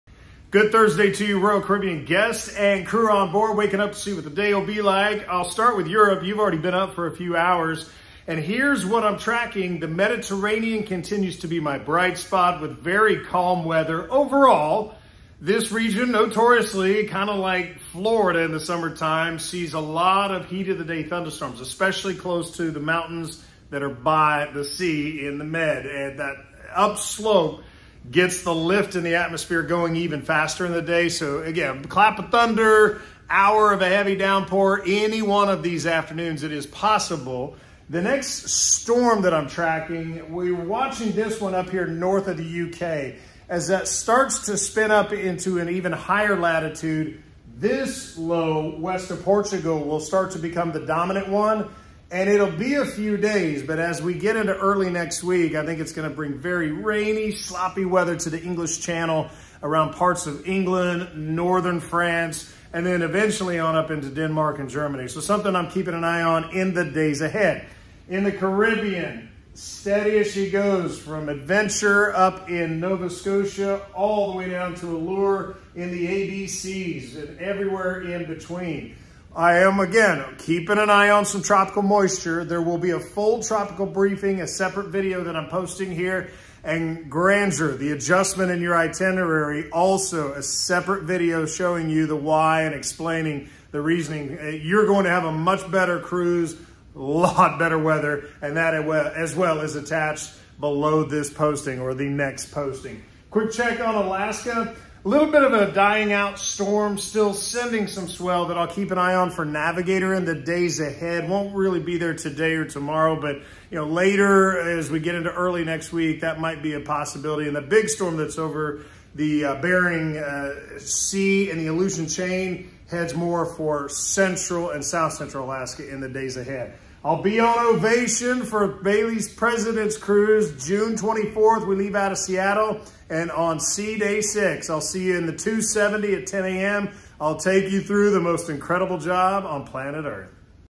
Happy Thursday World! Here’s your updated Forecast for the @RoyalCaribbean Fleet:
Happy Thursday World! Here’s your updated Forecast for the @RoyalCaribbean Fleet in Europe, Alaska, and the Caribbean for June 16th, 2022 (Grandeur there is a video just for you following this post, and a Tropical Briefing for everyone as the third posted video):